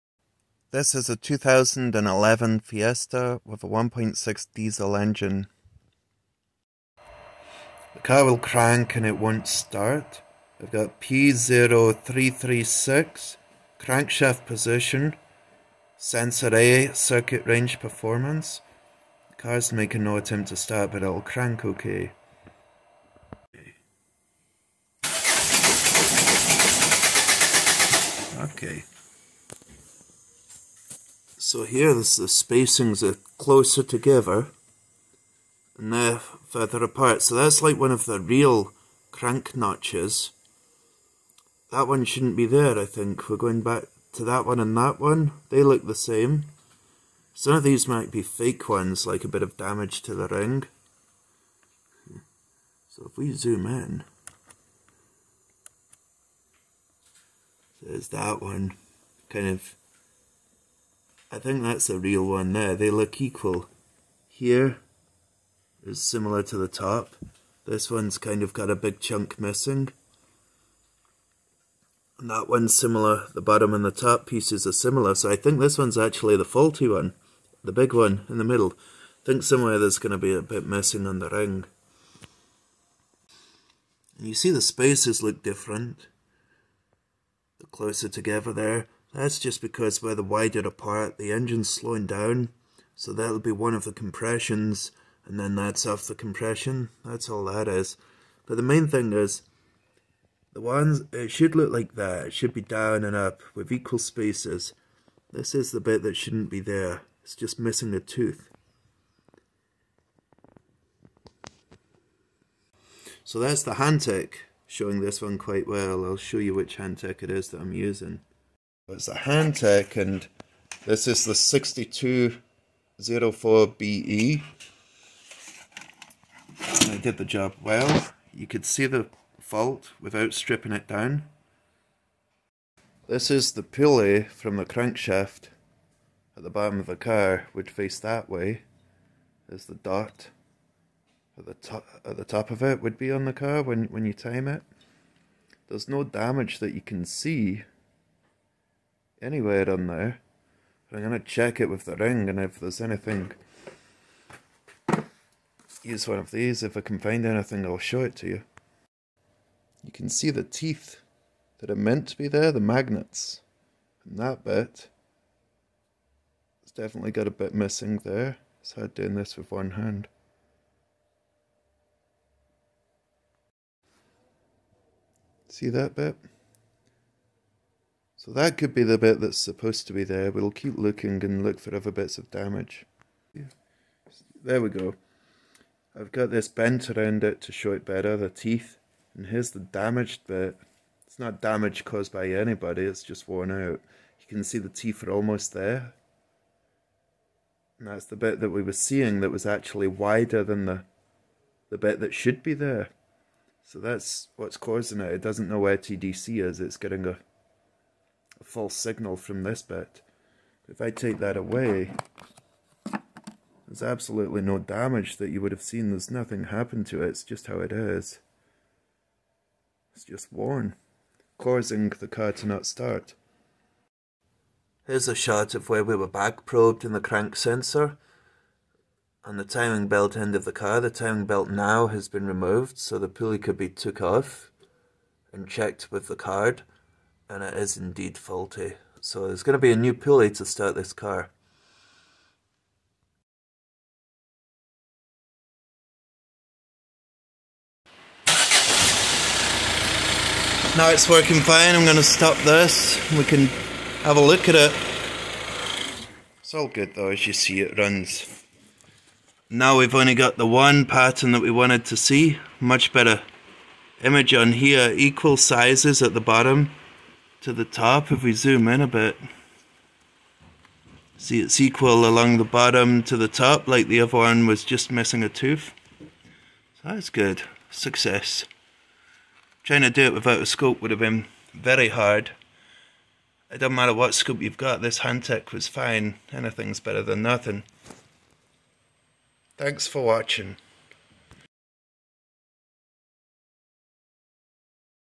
Ford Fiesta (crank no start) P0336 Crank sensor range/performance
Hi I'm Matt and in this video I am showing a Ford Fiesta 1.6 tdci with a P0336 crank sensor range/performance fault code, causing the car to crank and not start.
using an oscilloscope back probed at the crank sensor's signal wire I was able to see that the pick up ring had an extra flat spot on the ring.
this saved stripping down the crank to check timing
the crank pulley with the faulty pick up ring actually looked fine when looking at it by eye.
this one would have been hard to diagnose without an oscilloscope.